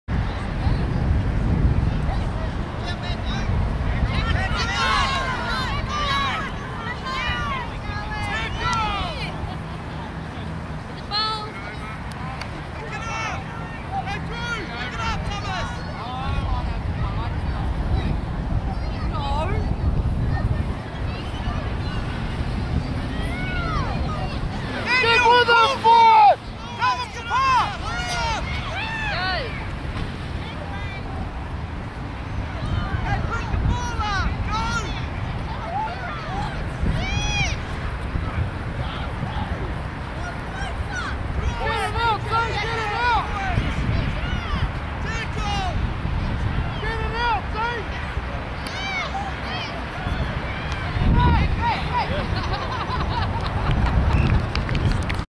Boston scores on his first appearance for the Souljahs
U11's Onepoto Souljahs vs Waikanae Force 5th June 2010

Boston scores on his first Souljahs appearance after some solid power from all of the forwards

Commentary and Camera Work by George :-)